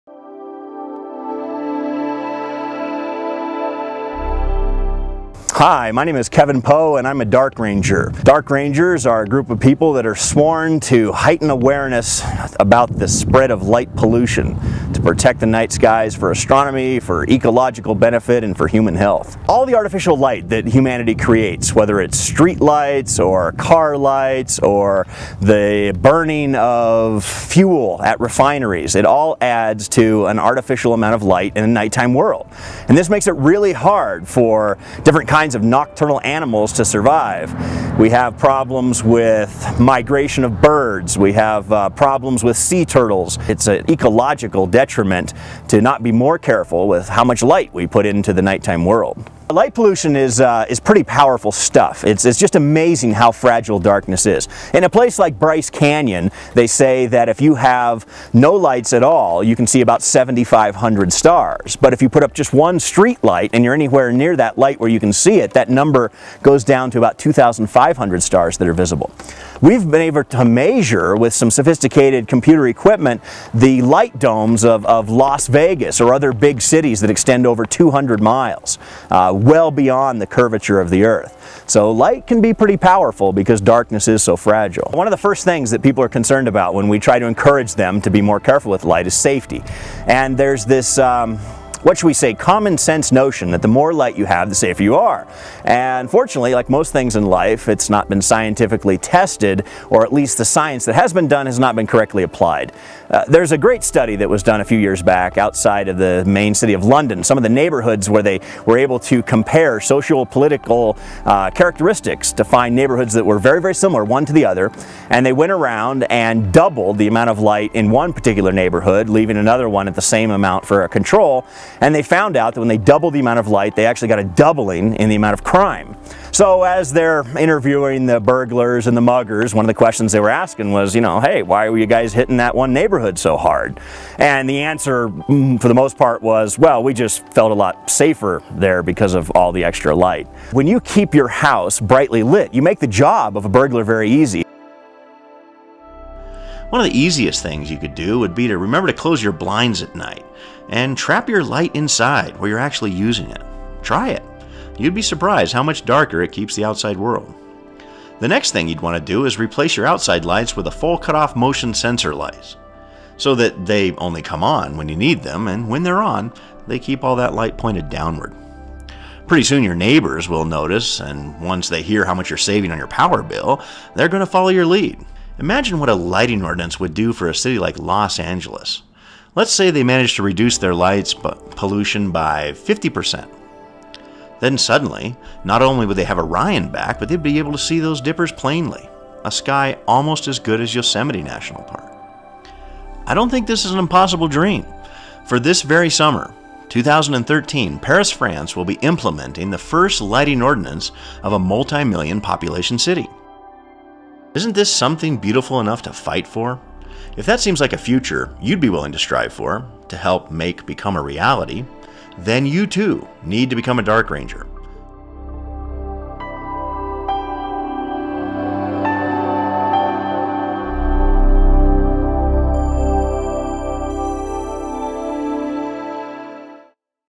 What is Light Pollution, and How Can You Help Reduce It?
Dark Ranger, Kevin Poe, talks about what light pollution is, and provides ways you can help reduce it.

Produced by - On the Leesh Productions
Starring - Dark Ranger Kevin Poe
Edited by - Amanda Blohm
What You Can Do Logo Created by: SCG Siddharth Creative Group

To find out more about Kevin Poe and how you can become a Dark Ranger, please visit: